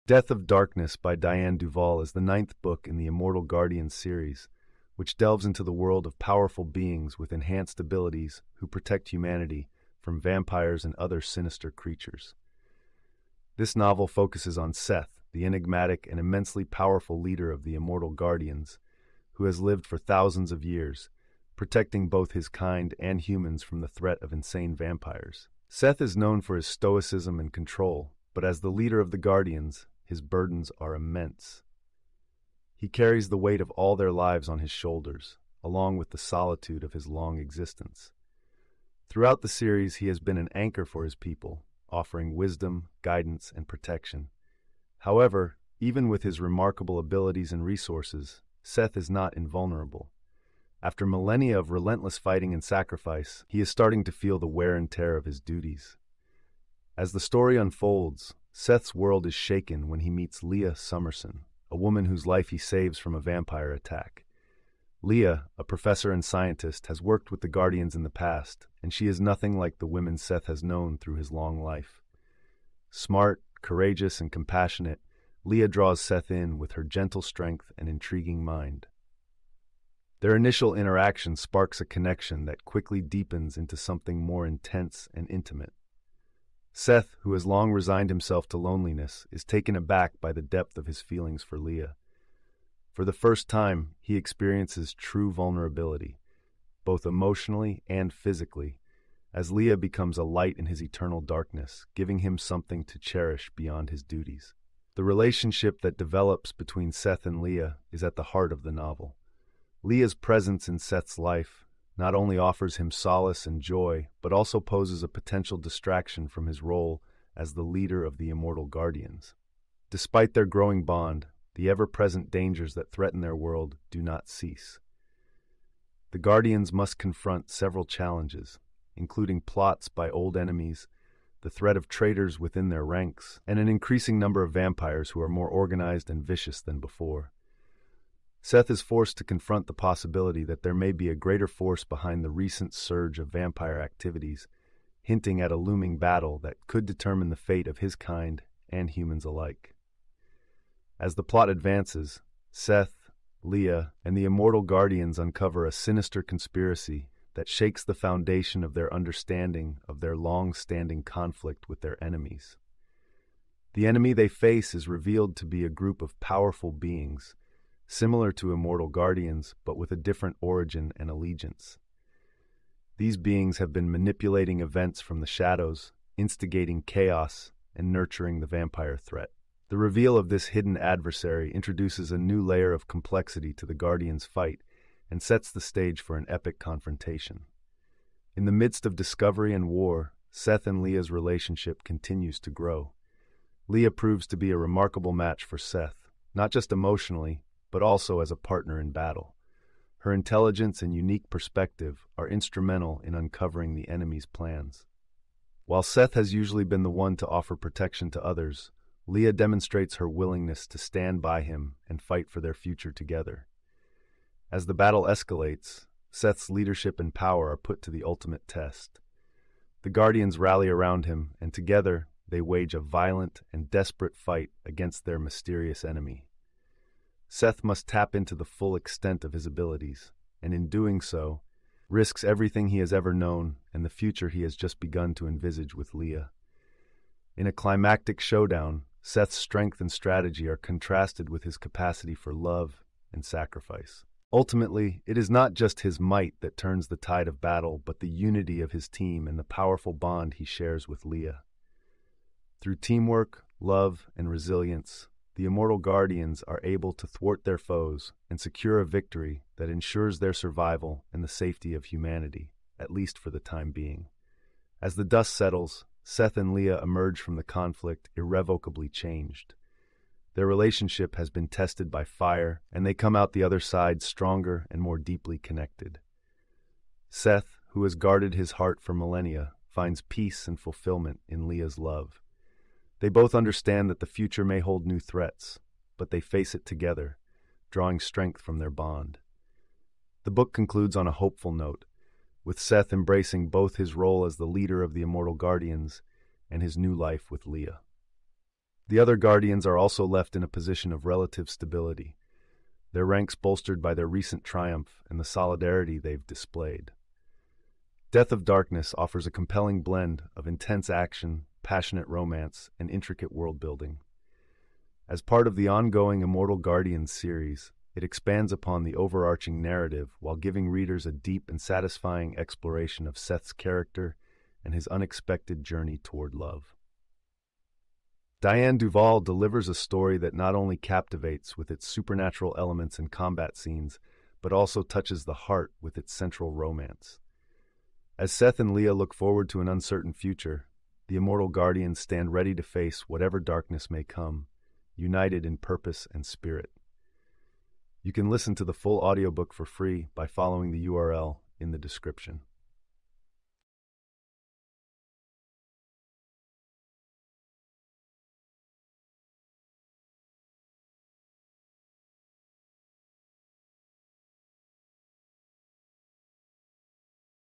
Death of Darkness: Immortal Guardians Series, Book 9
Get the Full Audiobook for Free:
"Death of Darkness" is the ninth installment in Dianne Duvall's Immortal Guardians series, in which Seth, the powerful and ancient leader of the guardians, finally meets his match in a woman named Leah and must confront a formidable enemy that threatens their world and their newfound love.